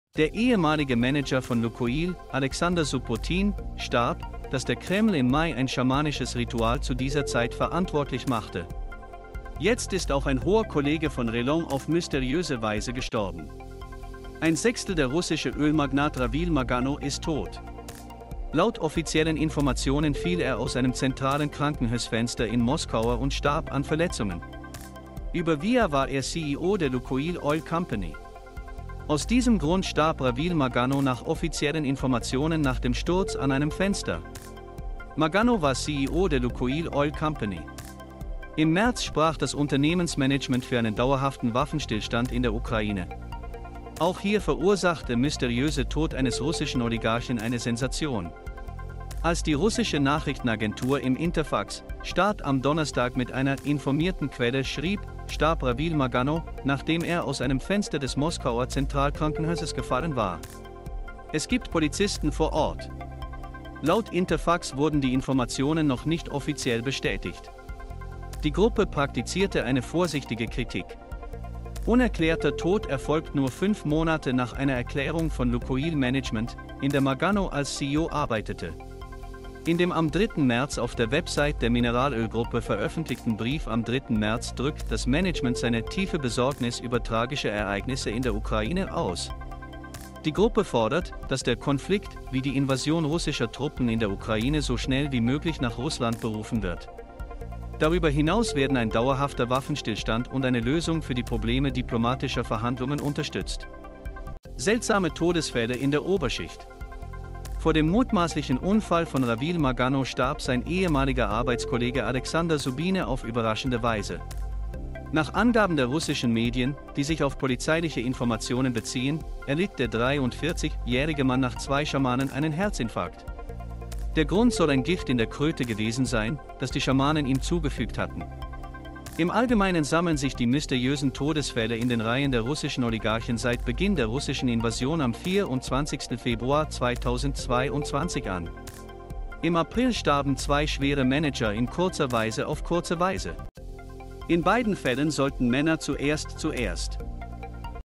Ravil Maganow: Kreml kritisierte Ölmagnat stirbt, nachdem er aus dem Fenster des Moskauer Krankenhau
Der ehemalige Manager von Lukoil, Alexander Subbotin, starb, dass der Kreml im Mai ein schamanisches Ritual zu dieser Zeit verantwortlich machte. Jetzt ist auch ein hoher Kollege von Relon auf mysteriöse Weise gestorben.
Der ehemalige Manager von Lukoil, Alexander Subbotin, starb, dass der Kreml im Mai ein schamanisches Ritual zu dieser Zeit verantwortlich machte. Jetzt ist auch ein hoher Kollege von Relon auf mysteriöse Weise gestorben. 1/6 Der russische Ölmagnat Ravil Maganow ist tot. Laut offiziellen Informationen fiel er aus einem zentralen Krankenhausfenster in Moskauer und starb an Verletzungen. Über via war er CEO der Lukoil Oil Company. Aus diesem Grund starb Ravil Maganow nach offiziellen Informationen nach dem Sturz an einem Fenster. Maganow war CEO der Lukoil Oil Company. Im März sprach das Unternehmensmanagement für einen dauerhaften Waffenstillstand in der Ukraine. Auch hier verursacht der mysteriöse Tod eines russischen Oligarchen eine Sensation. Als die russische Nachrichtenagentur im Interfax -Staat am Donnerstag mit einer "informierten Quelle" schrieb, starb Ravil Maganow, nachdem er aus einem Fenster des Moskauer Zentralkrankenhauses gefallen war. Es gibt Polizisten vor Ort. Laut Interfax wurden die Informationen noch nicht offiziell bestätigt. Die Gruppe praktizierte eine vorsichtige Kritik

Unerklärter Tod erfolgt nur fünf Monate nach einer Erklärung von Lukoil Management, in der Maganow als CEO arbeitete. In dem am 3. März auf der Website der Mineralölgruppe veröffentlichten Brief am 3. März drückt das Management seine "tiefe Besorgnis über tragische Ereignisse in der Ukraine" aus. Die Gruppe fordert, dass der "Konflikt" wie die Invasion russischer Truppen in der Ukraine so schnell wie möglich nach Russland berufen wird. Darüber hinaus werden ein dauerhafter Waffenstillstand und eine Lösung für die Probleme diplomatischer Verhandlungen unterstützt. Seltsame Todesfälle in der Oberschicht

Vor dem mutmaßlichen Unfall von Ravil Maganow starb sein ehemaliger Arbeitskollege Alexander Subbine auf überraschende Weise. Nach Angaben der russischen Medien, die sich auf polizeiliche Informationen beziehen, erlitt der 43 -jährige Mann nach zwei Schamanen einen Herzinfarkt. Der Grund soll ein Gift in der Kröte gewesen sein, das die Schamanen ihm zugefügt hatten. Im Allgemeinen sammeln sich die mysteriösen Todesfälle in den Reihen der russischen Oligarchen seit Beginn der russischen Invasion am 24. Februar 2022 an. Im April starben zwei schwere Manager in kurzer Weise auf kurze Weise. In beiden Fällen sollten Männer zuerst zuerst